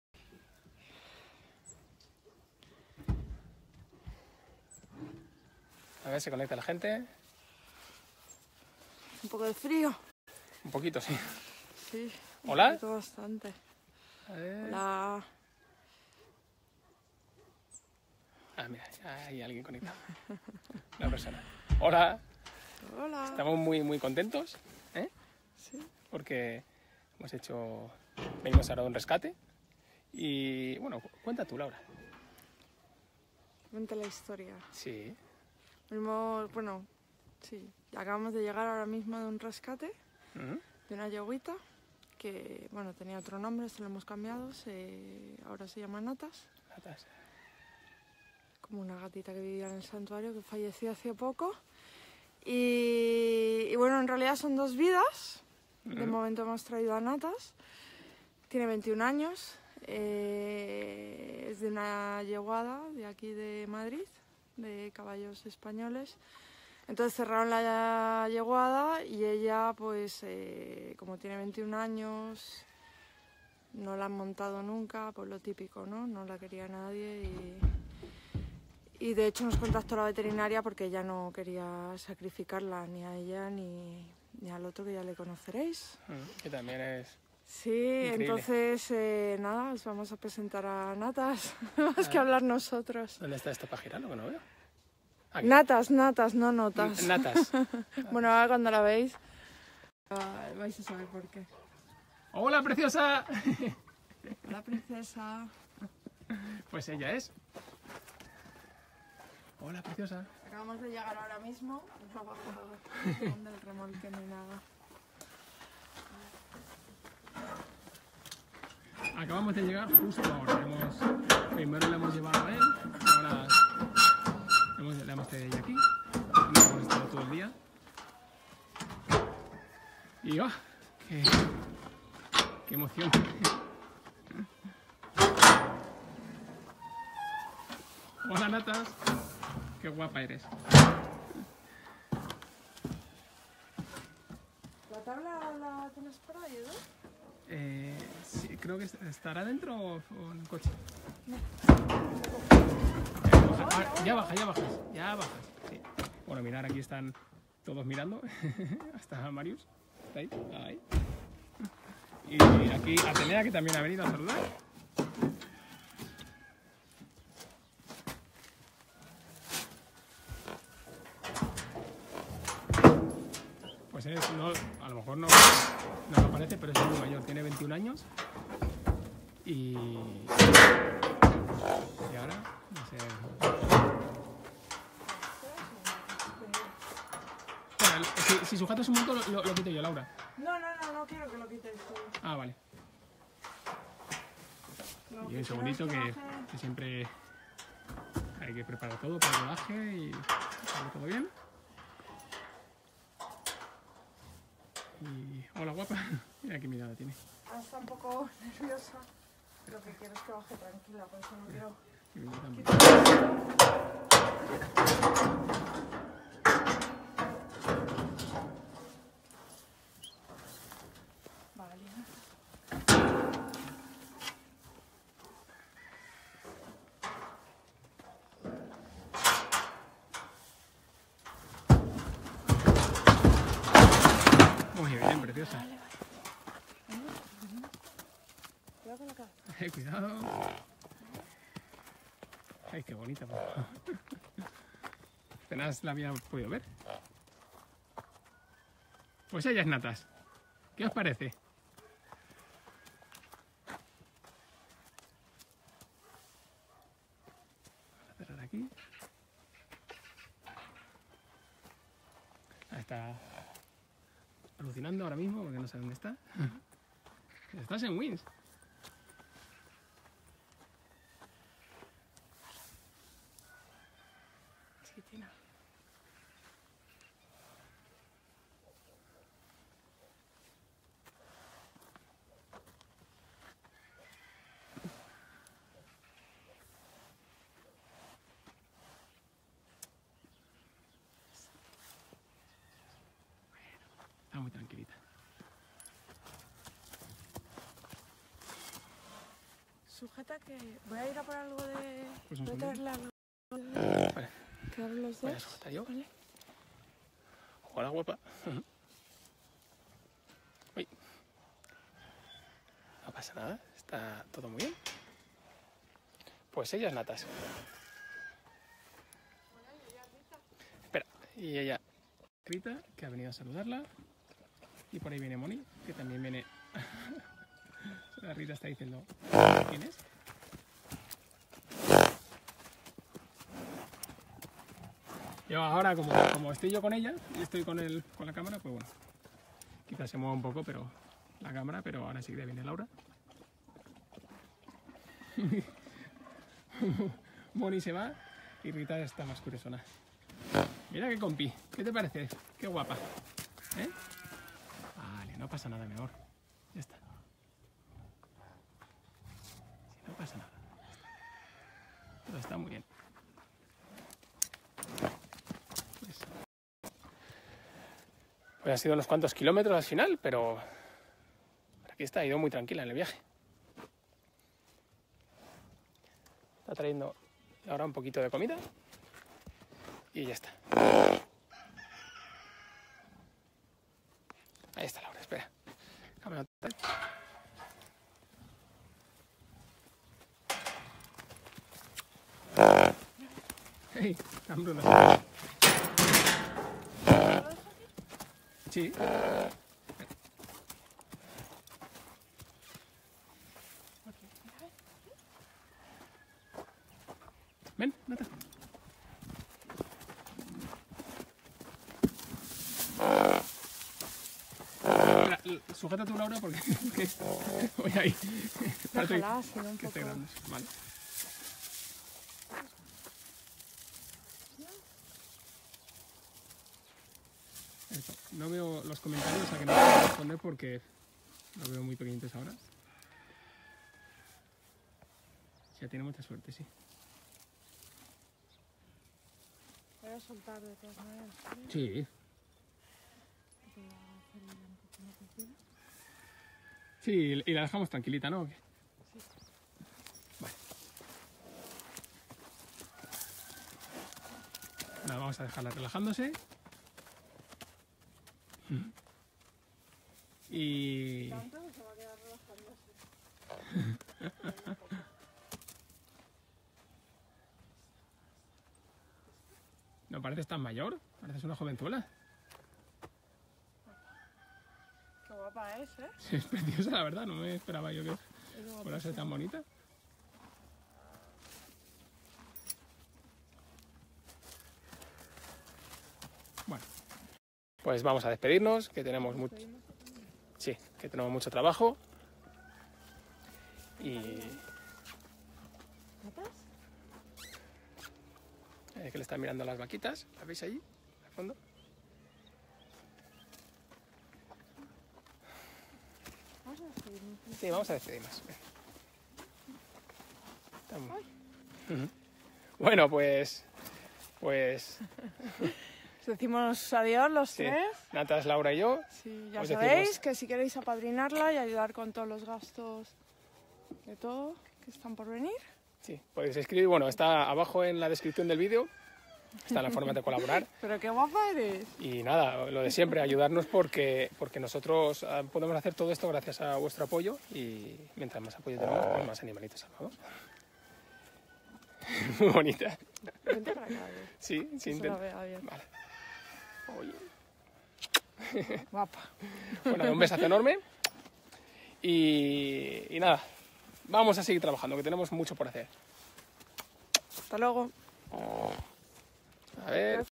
Rescate de una yegua de 21 años
Natas es una yegua ía que pertenecía a una yeguada de Madrid. La yeguada cerró y a ella la iban a sacrificar por no valer para ser montada.